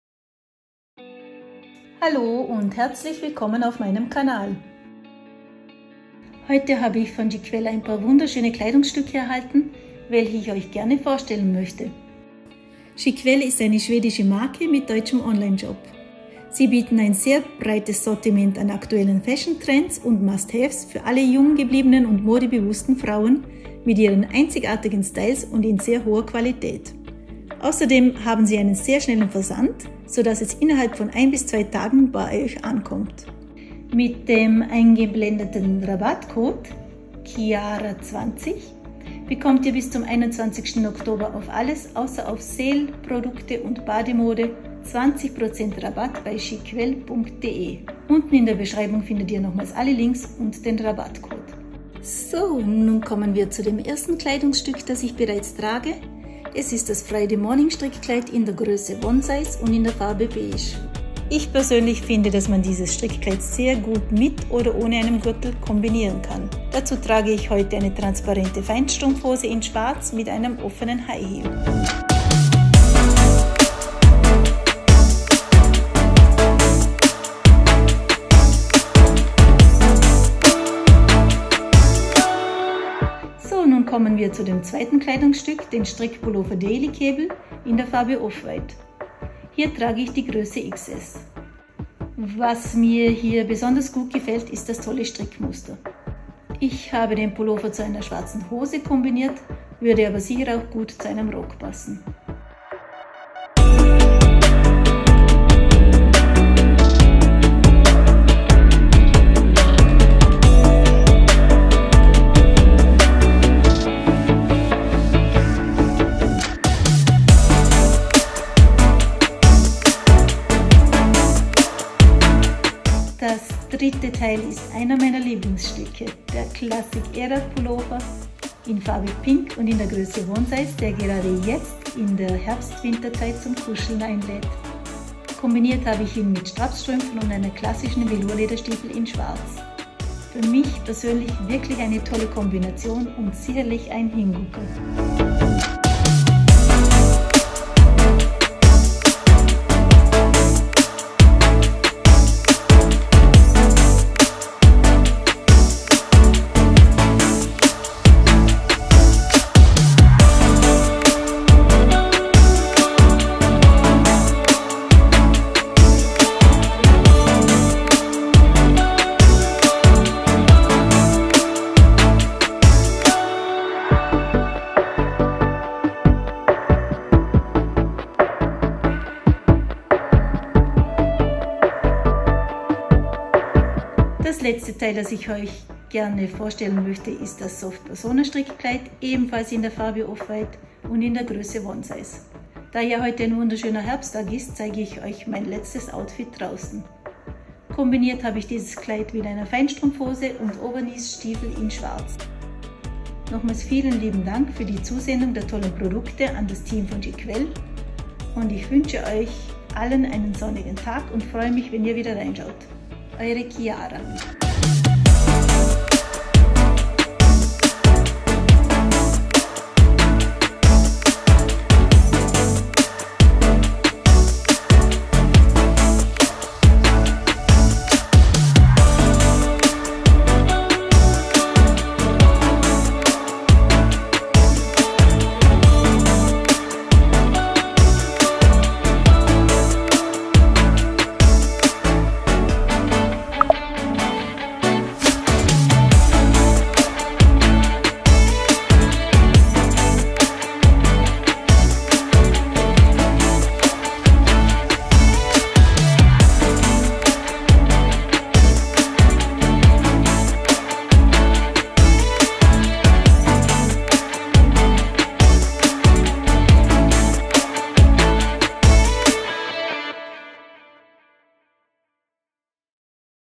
TRY ON HAUL CHIQUELLE - Herbst Kollektion, Mini dress, High Heels, Pantyhose, walking in public, 4K
Friday Morning Knit Dress
Farbe: Beige
Grösse: One Size

Daily Cable Knit
Farbe: Offwhite
Grösse: XS/S

Classic Era Knit
Farbe: Pink
Grösse: One Size

Soft Persona Knit
Farbe: Offwhite
Grösse: One Size

Ich freu mich über weitere Vorschläge oder Ideen, lasst mir dazu einfach ein Kommentar da.

Danke für den lieben Support
Kyara